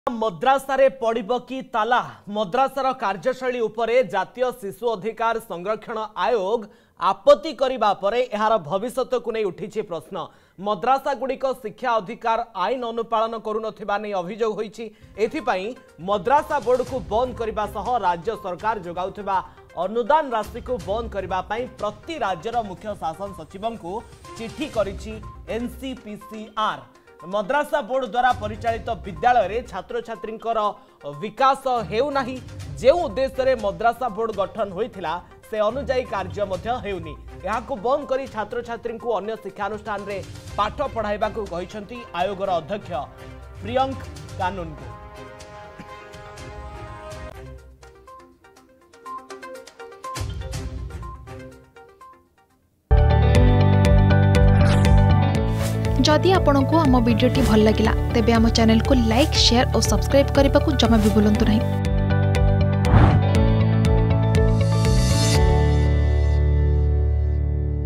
ମଦ୍ରାସାରେ ପଡ଼ିବ କି ତାଲା ? | Madarasas Issue
ମଦ୍ରାସାରେ ପଡ଼ିବ କି ତାଲା ? ମଦ୍ରାସାର କାର୍ଯ୍ୟଶୈଳୀ ଉପରେ ଜାତୀୟ ଶିଶୁ ଅଧିକାର ସଂରକ୍ଷଣ ଆୟୋଗ ଆପତ୍ତି କରିବା ପରେ ଏହାର ଭବିଷ୍ୟତକୁ ନେଇ ଉଠିଛି ପ୍ରଶ୍ନ ।